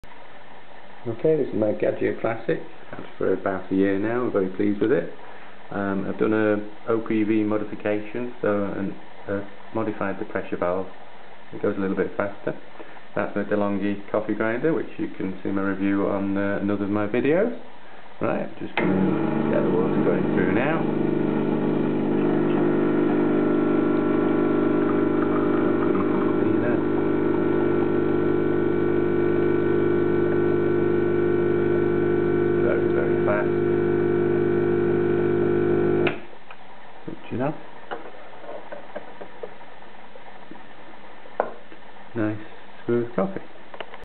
Gaggia Classic OPV Mod Speed Increase pressure Over Pressure Valve modified
**Gaggia Classic Coffee Machine I changed the OPV Adjustment on my Gaggia Classic which has increased the pressure and speed of making an espresso (or ristretto).**Check out my other coffee maker reviews and videos.